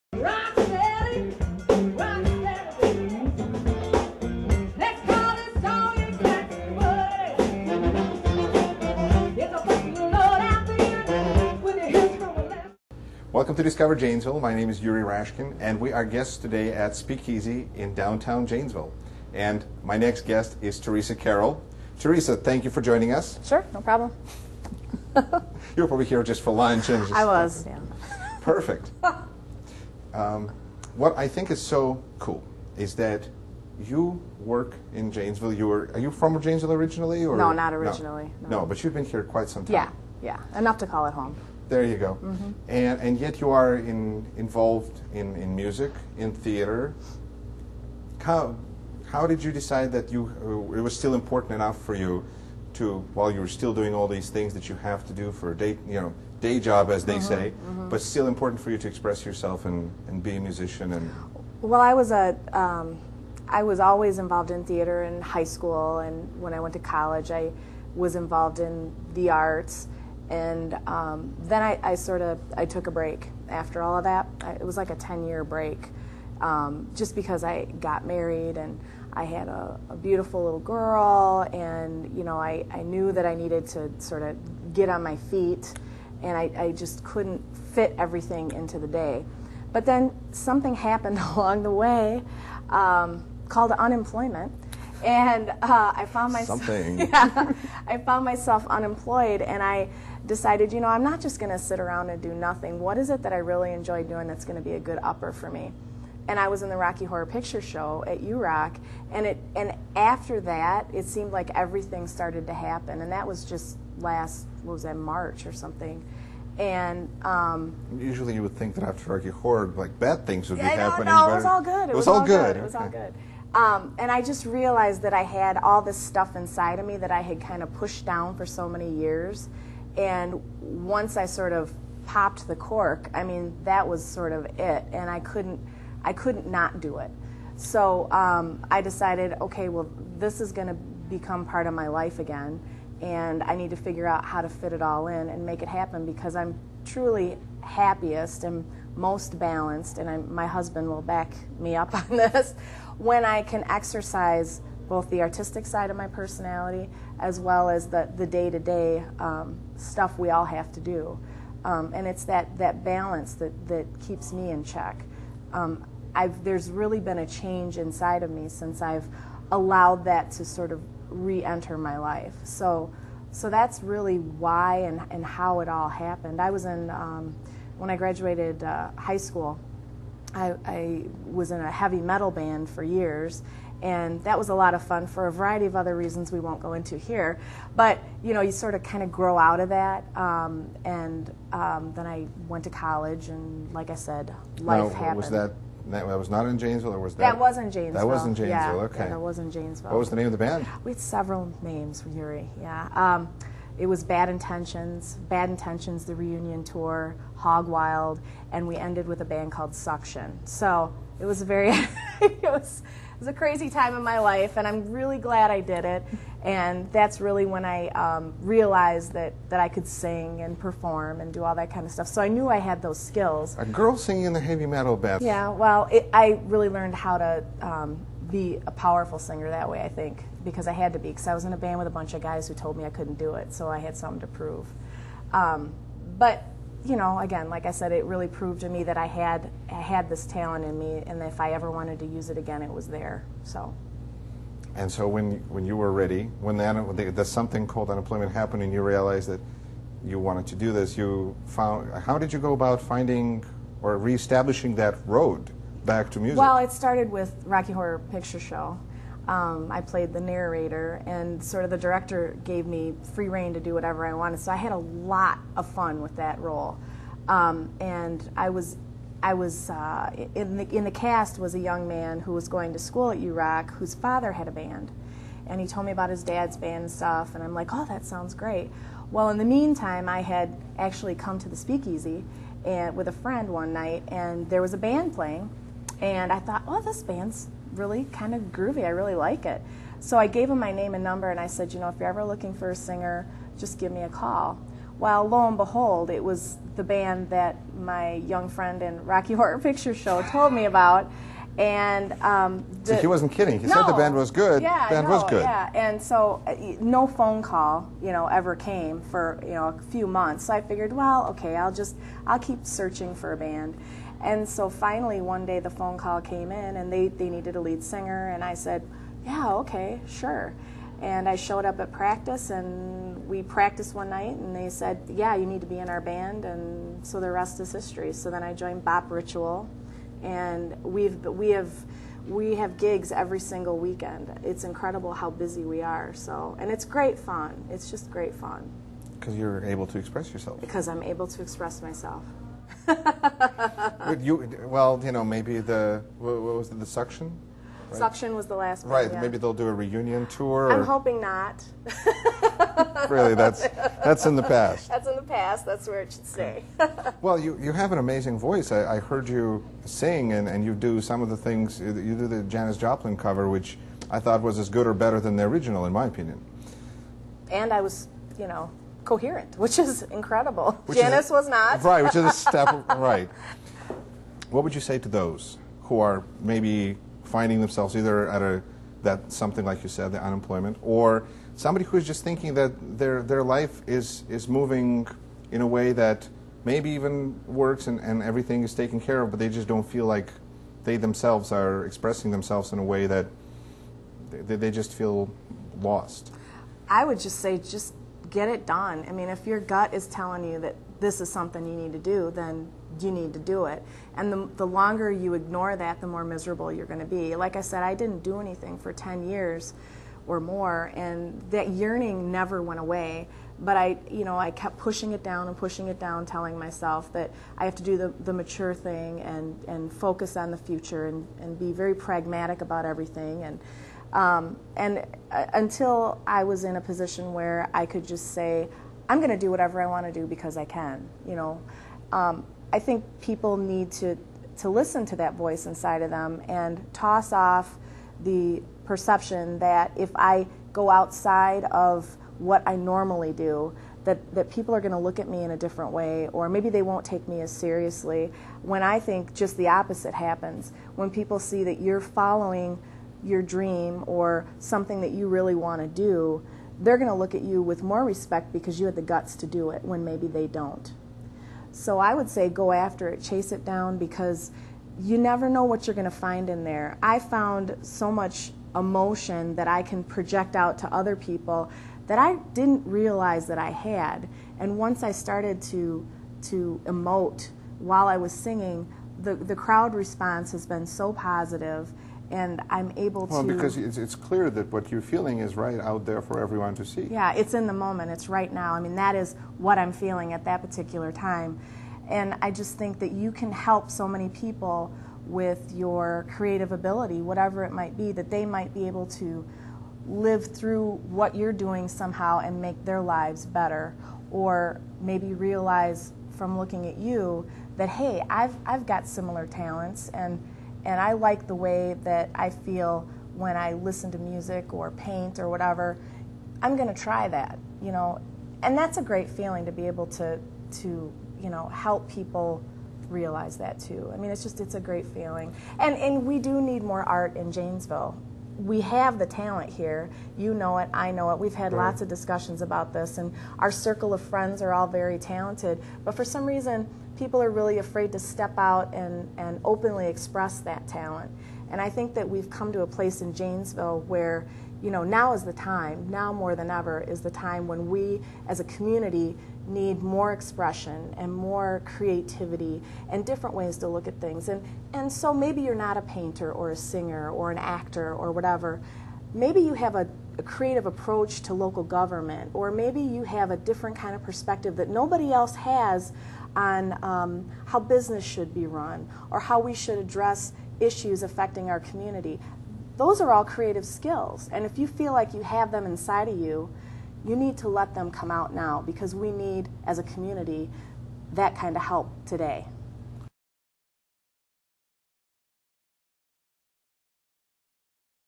Discover Janesville with Yuri Rashkin - Theresa Carroll - part 1
Interview with Theresa Carroll who has a day job with Bliss Communications and also sings with a band - Bop Ritual - at night.  Theresa and I talked about the value of music and self-expression to one's life and the importance of having the guts to step up and do what you love.